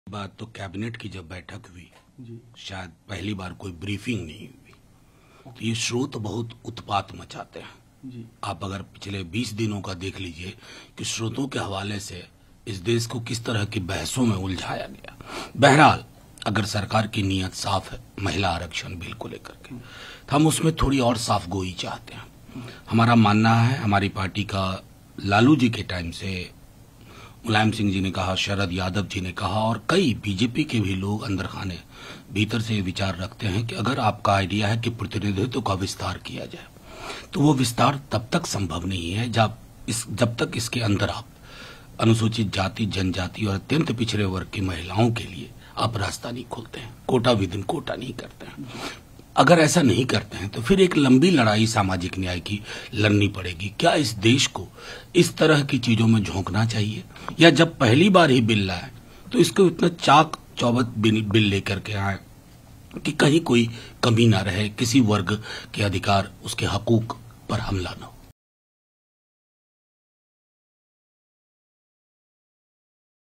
Women Reservation Bill पर Manoj Jha ने की 'फ्रेश बिल' की मांग
राजद नेता और सांसद मनोज झा (Manoj Jha) ने महिला आरक्षण विधेयक (Women Reservation Bill) पर बोलते हुए कहा कि अगर सरकार विधेयक का 2010 संस्करण ला रही है तो नया विधेयक लाएं और सुनिश्चित करें कि नए विधेयक में एससी और एसटी महिलाओं के लिए कोटे के भीतर कोटा हो.